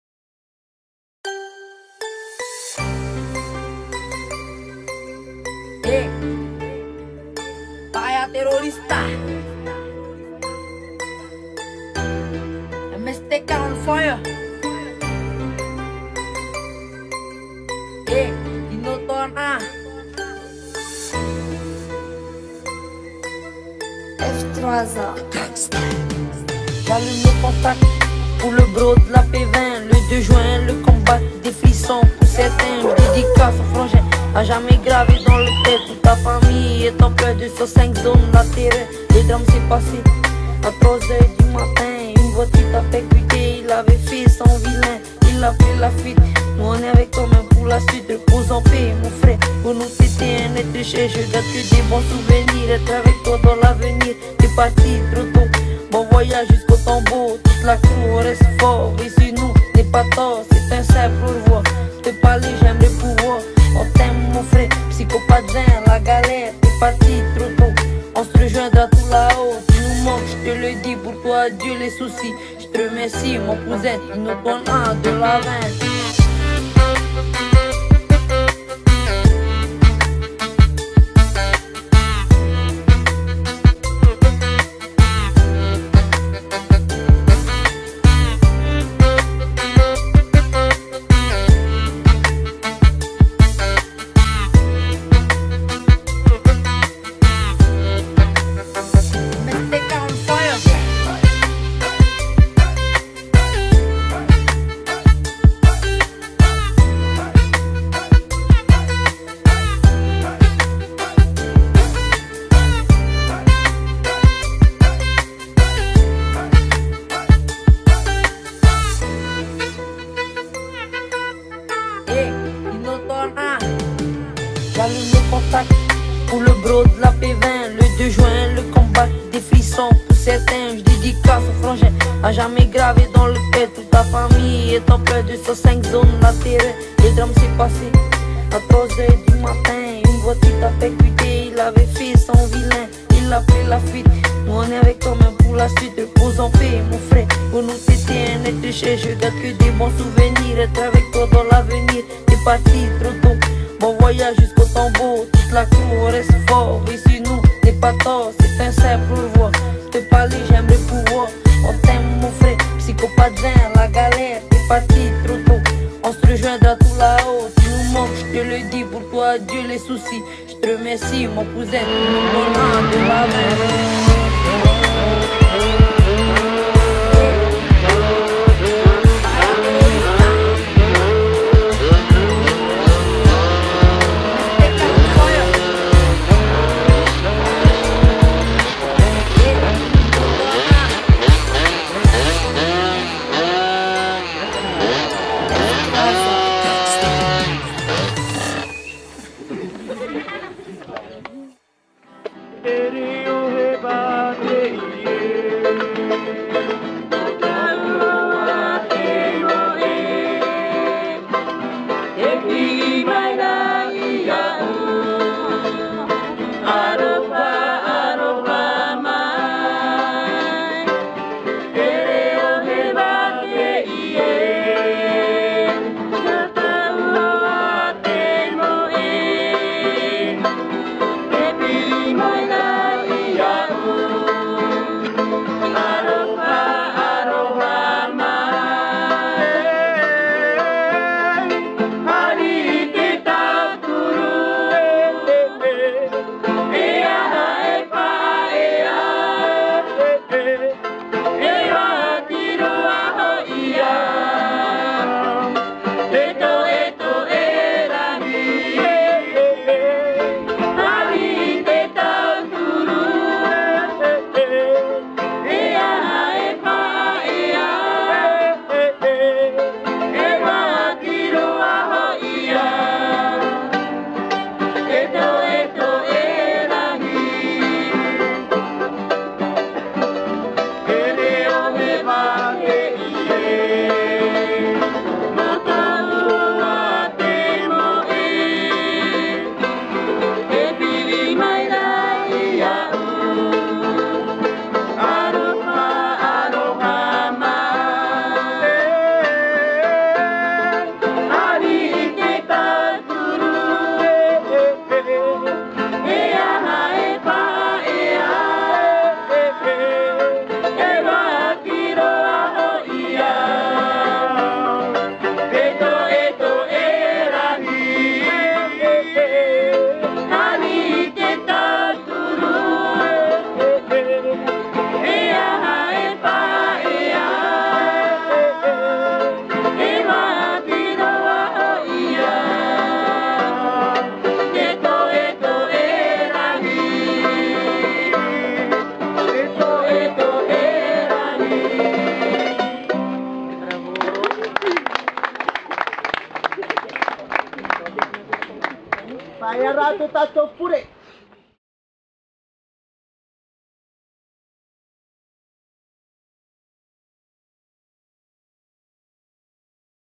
Hommage à Tinau Tuanaa (2001-2019)
A la douce mémoire de Tinau Willy Tuanaa, mon jeune collègue de 18 ans fauché par une voiture alors qu'il rentrait à vélo.

Le 3 juin 2019, vers 3h du matin, alors qu'il se rend au marché de Papeete pour y travailler, un automobiliste qui avait bu 7 obus de bière ce soir-là, percute un vélo dans le sens contraire de la circulation, au niveau de Tiapa, à Paea (Tahiti).
Sur le vélo, deux amis de 17 et 18 ans. Le plus âgé, Tinau, décède sur place. L'autre est hospitalisé avec 45 jours d'ITT.